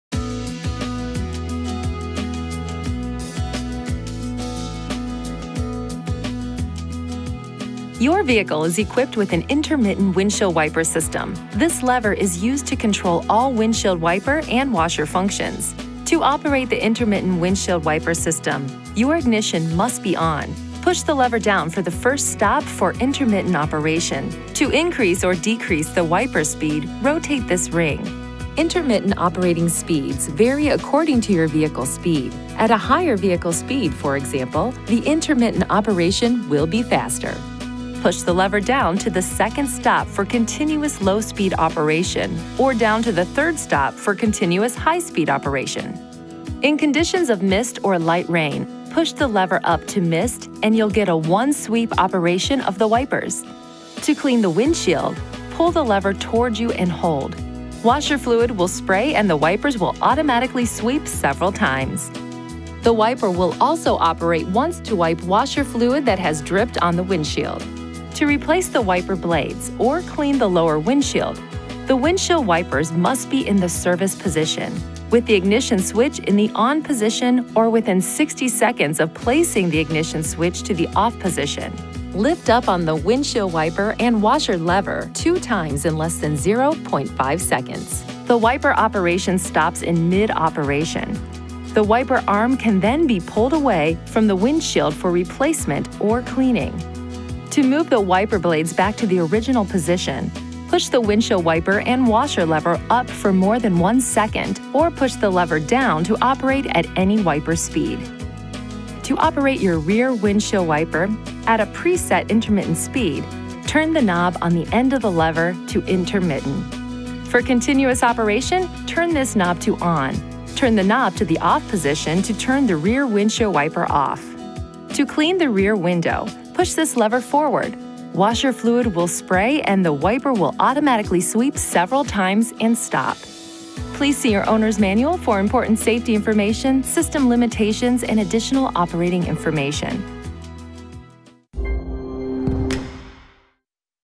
Nissan – Windshield Wiper and Washer Controls
You will find quick reference guides and other useful information about your warranty, Bluetooth® system, and much more!

Your vehicle is equipped with an intermittent windshield wiper system. This lever is used to control all windshield wiper and washer functions
To operate the intermittent windshield wiper system, your ignition must be ON.

Push the lever down to the first stop for intermittent operation. To increase or decrease the wiper speed, rotate this ring.

Intermittent operating speeds vary according to your vehicle speed. At a higher vehicle speed, for example, the intermittent operation will be faster.

Push the lever down to the second stop for continuous low-speed operation or down to the third stop for continuous high-speed operation.

In conditions of mist or light rain, push the lever up to MIST and you’ll get a one-sweep operation of the wipers.

To clean the windshield, pull the lever toward you and hold. Washer fluid will spray and the wipers will automatically sweep several times.

The wiper will also operate once to wipe washer fluid that has dripped on the windshield.

To operate your rear windshield wiper at a preset intermittent speed; turn the knob on the end of the lever to INTERMITTENT.

For continuous operation, turn this knob to ON. Turn the knob to the OFF position to turn the rear windshield wiper OFF.

To clean the rear window, push this lever FORWARD. Washer fluid will spray and the wiper will automatically sweep several times and stop.